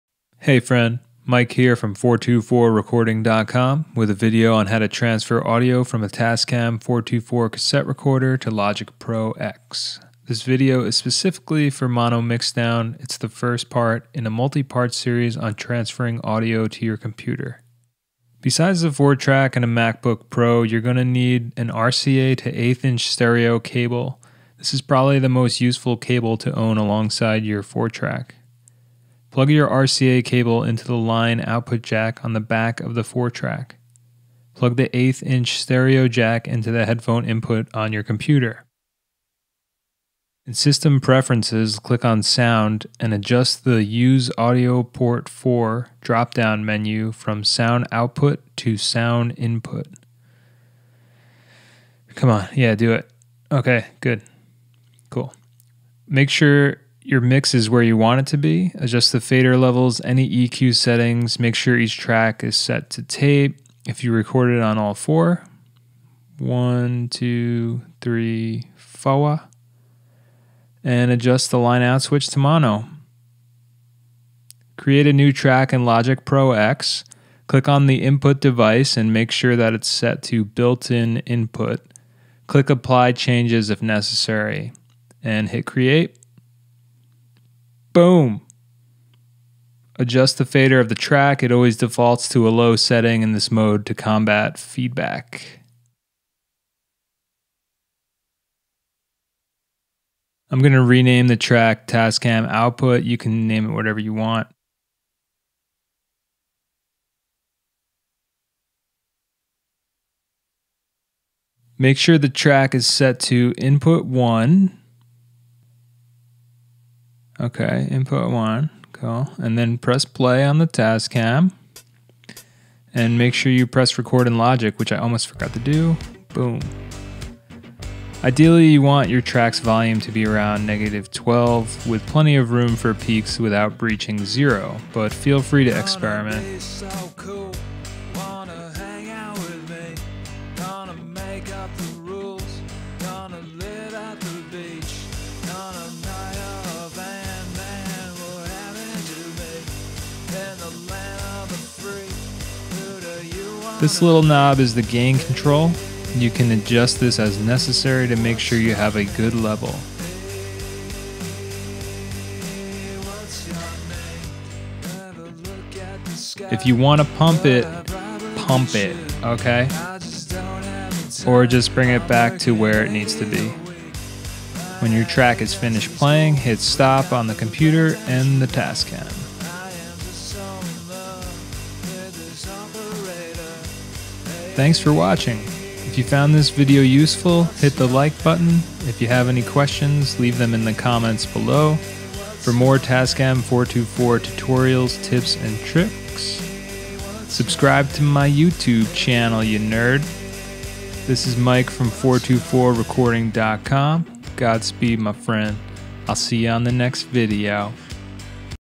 TASCAM 424: 4-TRACK AUDIO TRANSFER to Logic Pro X | 424recording.com
This is the first part of a multipart series of videos on how to transfer audio from a Tascam 424 portastudio 4-track cassette recorder to Logic Pro X. This video focuses on mono mixdown. The same, general principles should apply to other computer setups and DAWs.

At the time I made this video, I didn't realize that newer MacBook Pros do not have the audio input option. Here's an updated video regarding bouncing